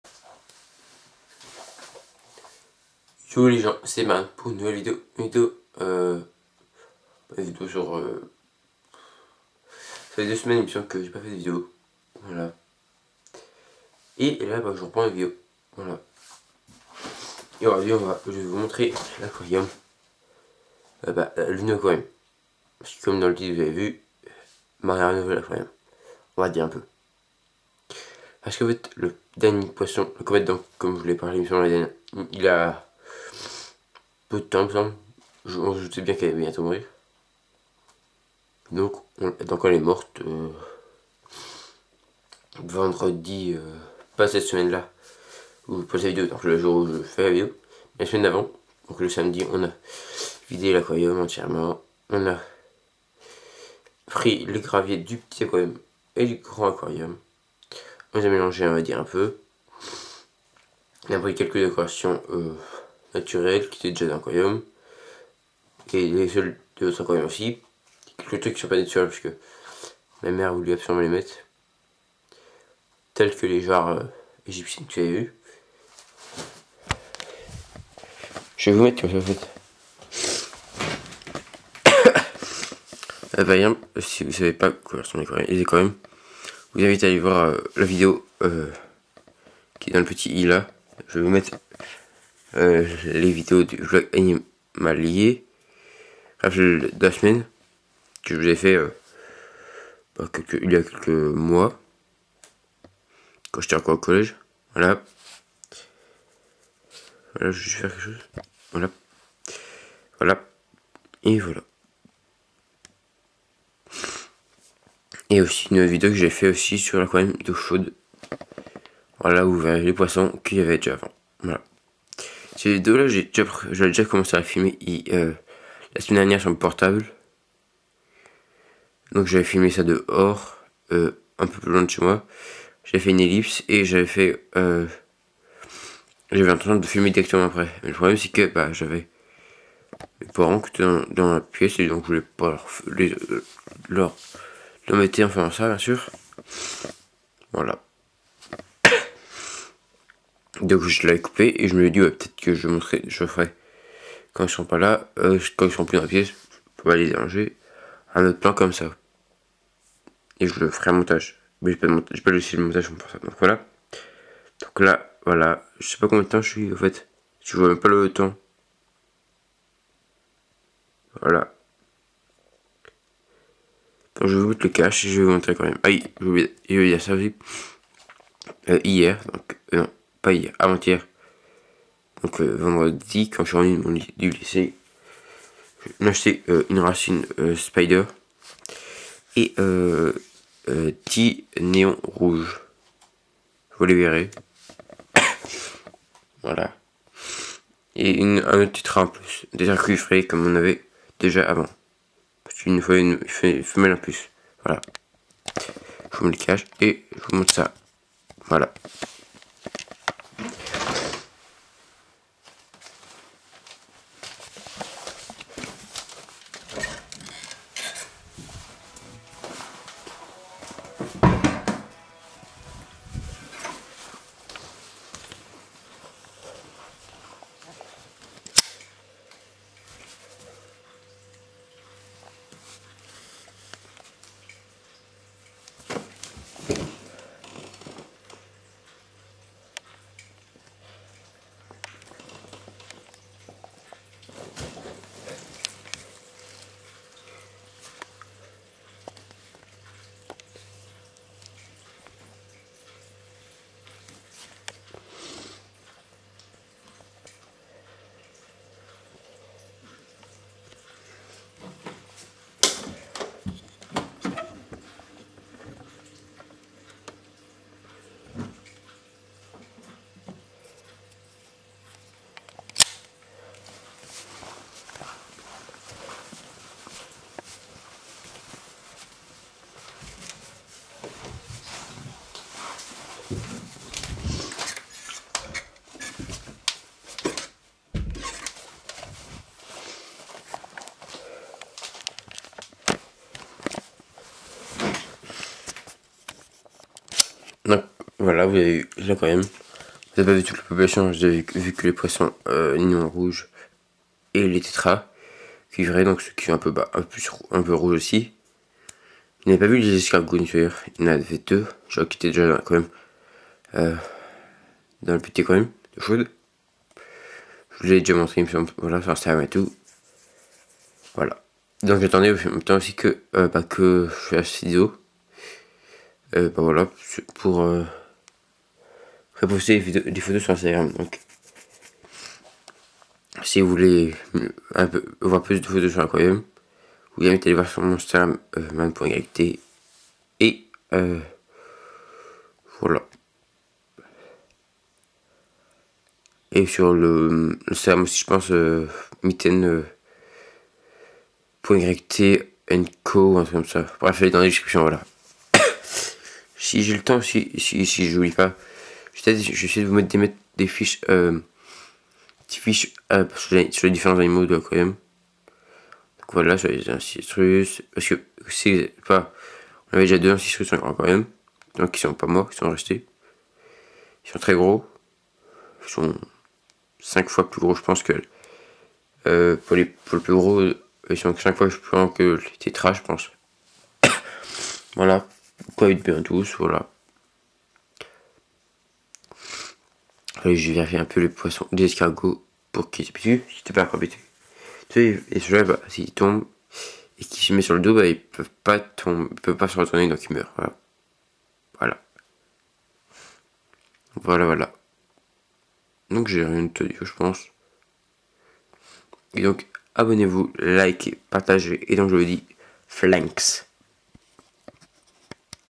🐚 Rénovation de l'aquarium de 120 L 🐟
Hey les gens c'est Marne Aujourd'hui je vais vous montrée la rénovation du grand aquarium  .

Espèce animal  présente:
- 6x tétra cuivre
- 2x ancistrus noire
-1x ancistrus doré
- 2 x neretina natalensis
-10 x néon rouge (cardinalis)

marne.yt
marne_my_drawing_dessin
mitaine.yt.andco
photographythemarne_

marne.yt

Mon SERVEUR discord principale

Mes Autre Chaîne YouTube

Si vous avez tous lu écrivez le Flanx .